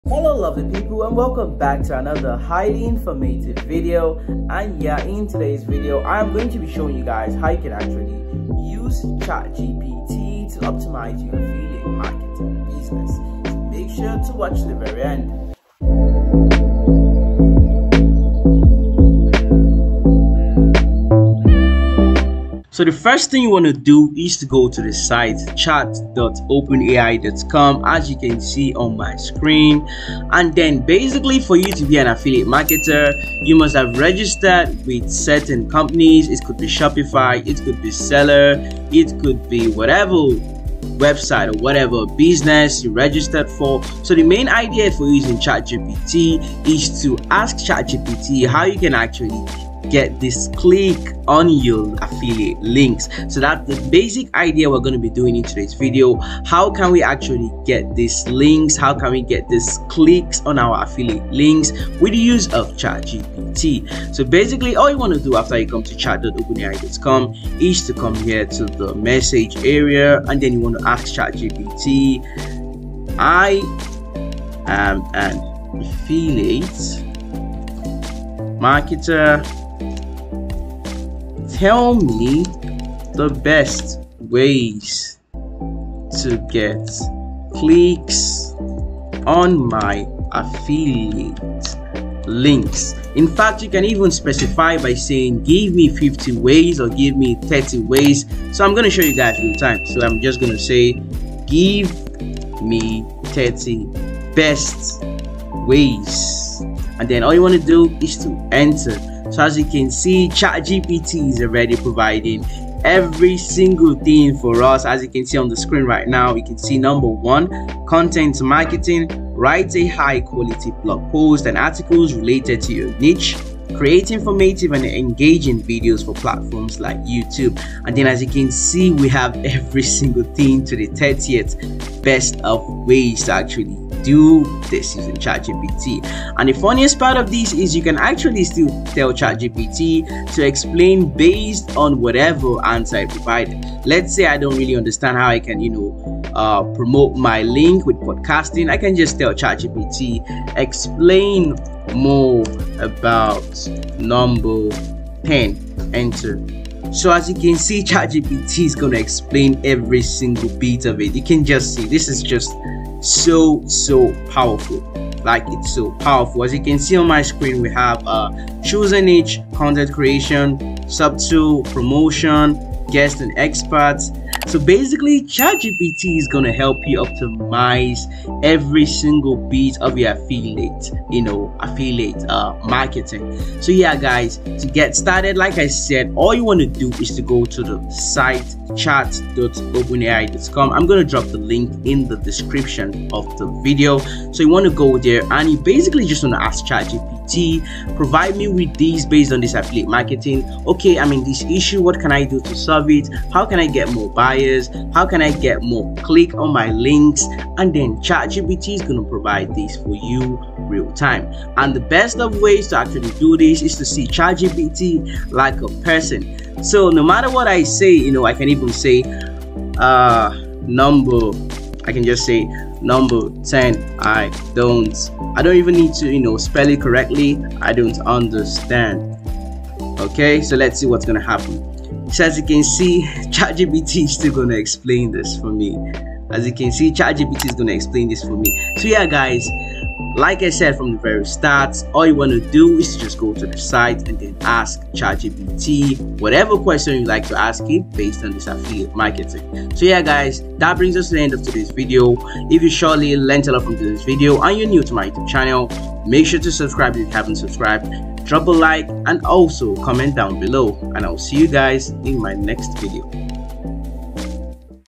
How To Use Chat Gpt For Affiliate Marketing | Step By Step Tutorial
In this video, You would learn all about How to use chat gpt for AFFILIATE MARKETING. This video explains all about using chat gpt to boost your affiliate marketing and how to get more clicks on Affiliate Links . This video Explains All you need to know in getting started with affiliate marketing using Chat Gpt. How to use chat gpt for affiliate marketing for beginners.

Make sure to watch to the end so as not to miss out on any important aspect of the video